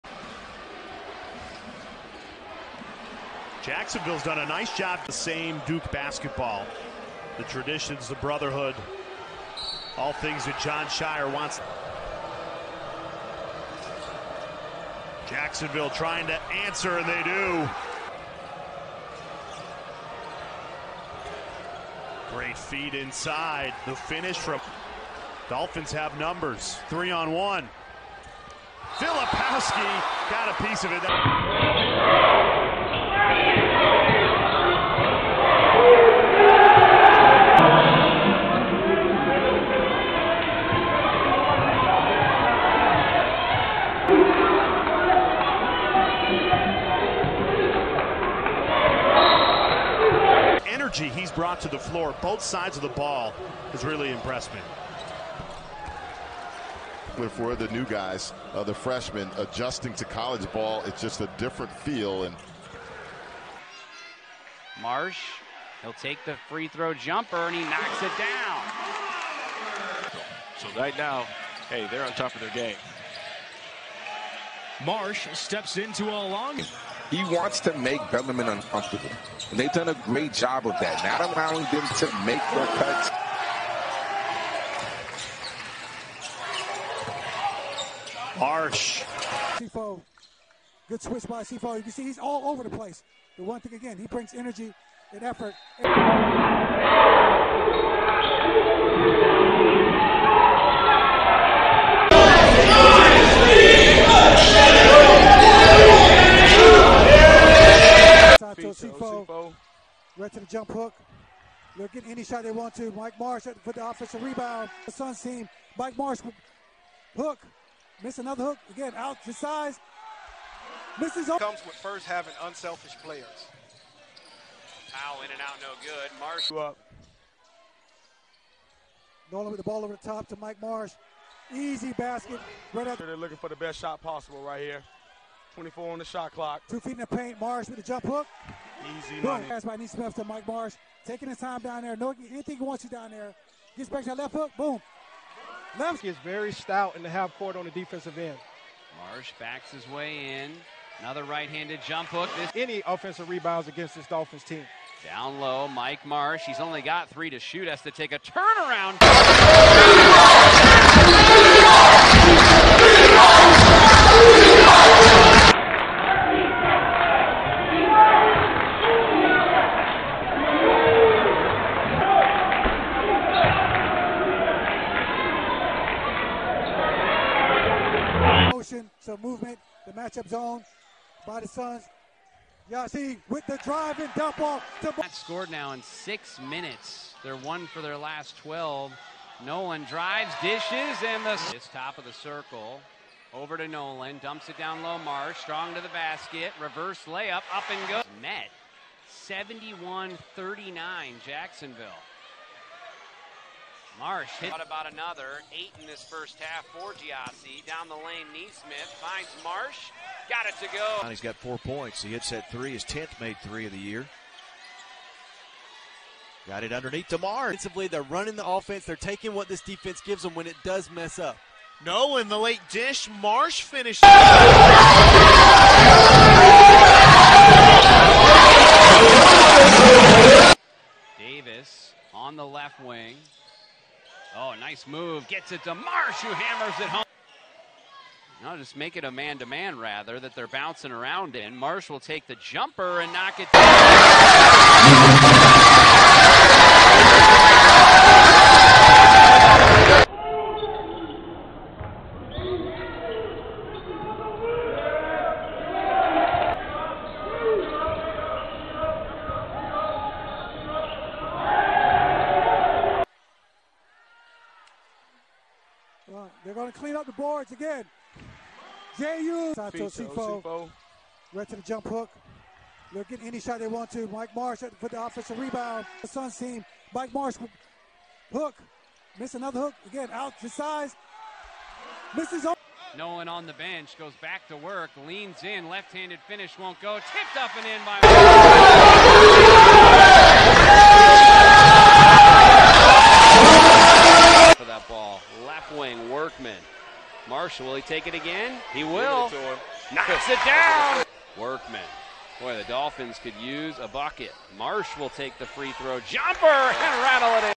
Mike Marsh - Post up big - Jacksonville - 2022-23 Transfer Portal Highlights
Headed to Oklahoma State!